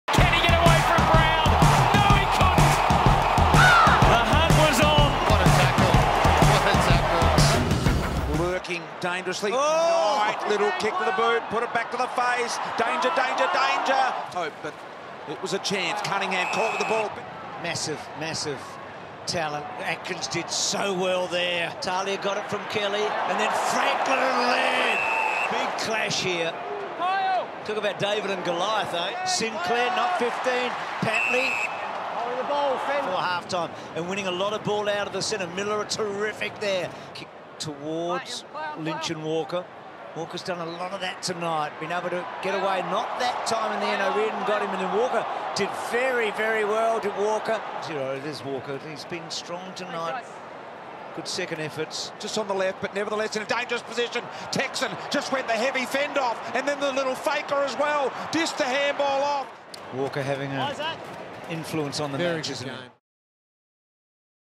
RAA Plays of the Week: R2 v Sydney
See all the best plays from our round 2 clash against Sydney.